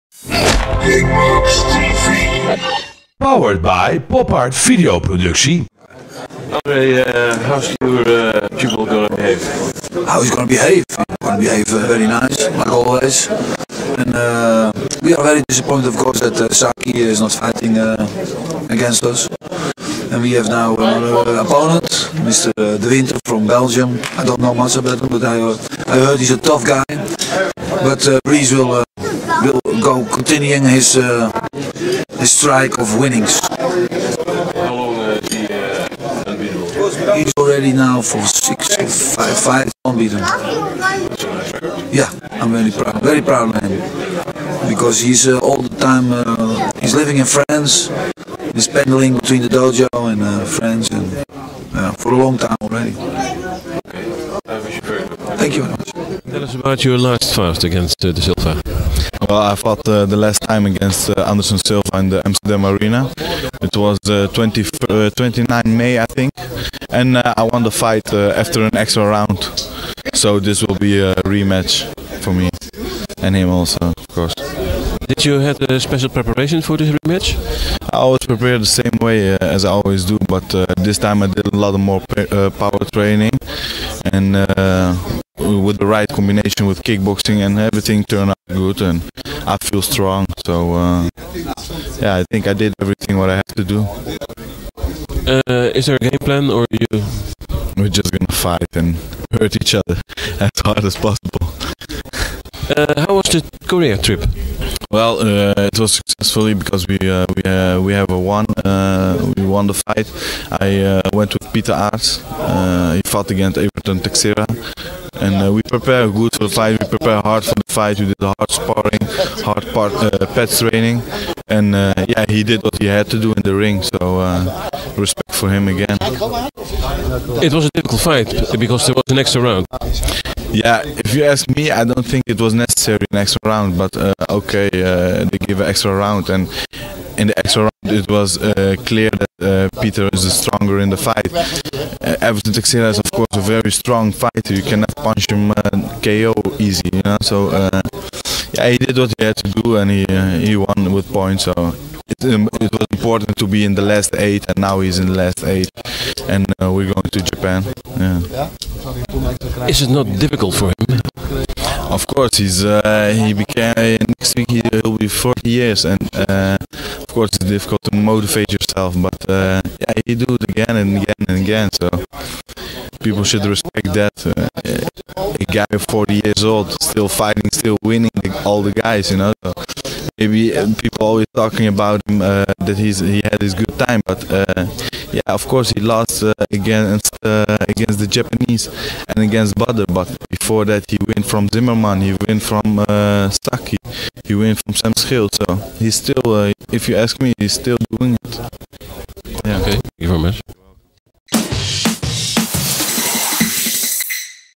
Interview with Andre Mannaart en Mourad Bouzidi
Interview with Andre Mannaart en Mourad Bouzidi @ Glory World Series Passenger Terminal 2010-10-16 in Amsterdam